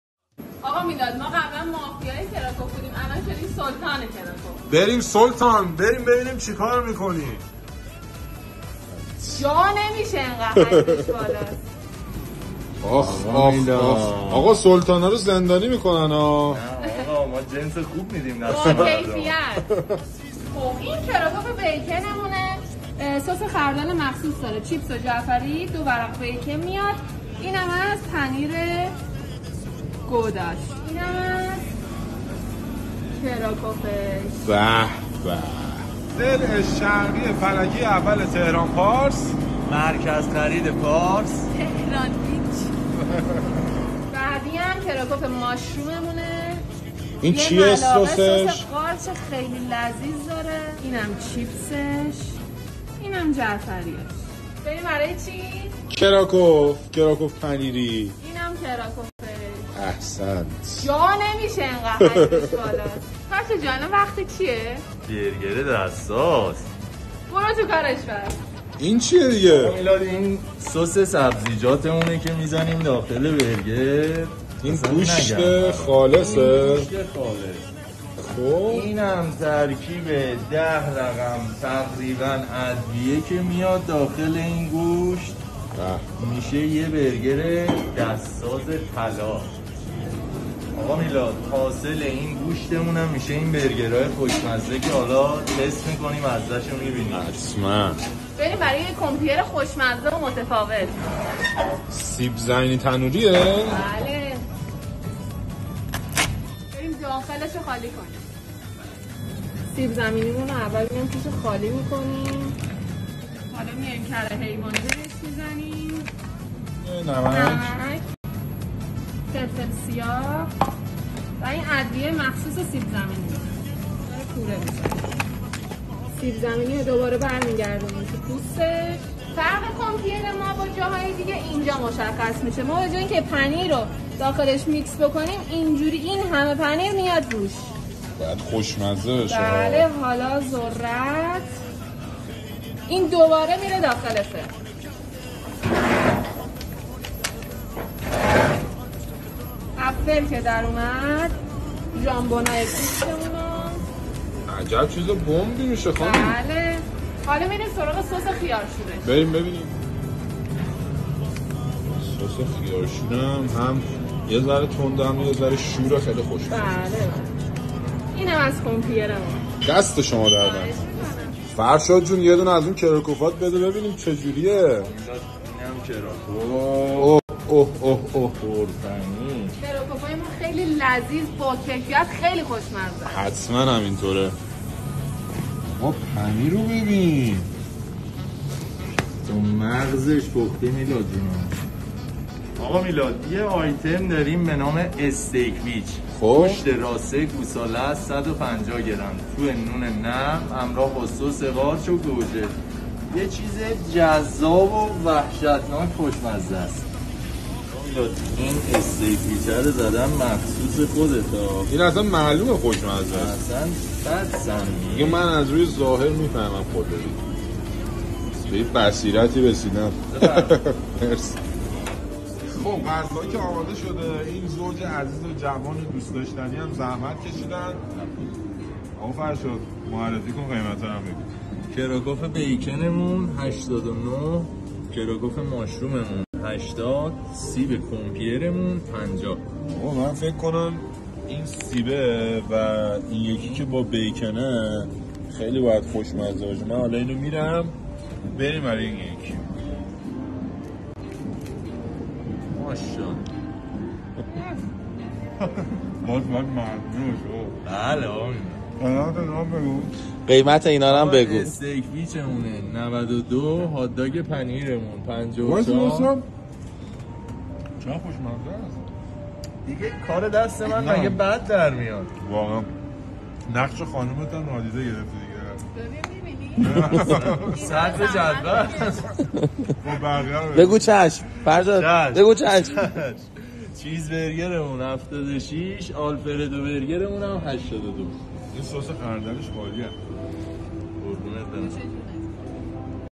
بهترین "کراکف" ایران را کجا داره؟😱
milad_taster's profile picture
milad_taster
این شما و این مافیا و سلطان کراکف تهران😋
این کراکف پنیریا که دیدید، خیلی پیشنهاد میشه. یعنی عالیه‌ها☺️ خصوصا با سس‌های مخصوصش که خردل و سس خیارشور هست خیلی خیلی لذیذ میشه🤤
اینجا یه فست‌فود دنج و باحال توی فلکه اول تهرانپارس هست که توسط یه زوج جوان راه‌اندازی و اداره میشه. از همون لحظه ورود برخورد خیلی صمیمی با مشتریها دارن و کلا هرکی میاد با لب خندون میره بیرون🤗
چند مدل کراکف پنیری، همبرگر و هات داگ دارن، ولی اون کمپیر سیب‌زمینی هم خیلی چیز کره‌ایه و حتما پیشنهادش می‌کنم🤤

داخل فست‌فود و طبقه بالا جا برای نشستن دارن، دم‌در هم میز و صندلی با گرماتاب گذاشتن که میتونید استفاده کنید👌

آدرس:
تهرانپارس، ضلع شرقی میدان، داخل محوطه مرکز خرید پارس، پلاک ۸ (ارسال به کل تهران هم دارن)

تلفن سفارش:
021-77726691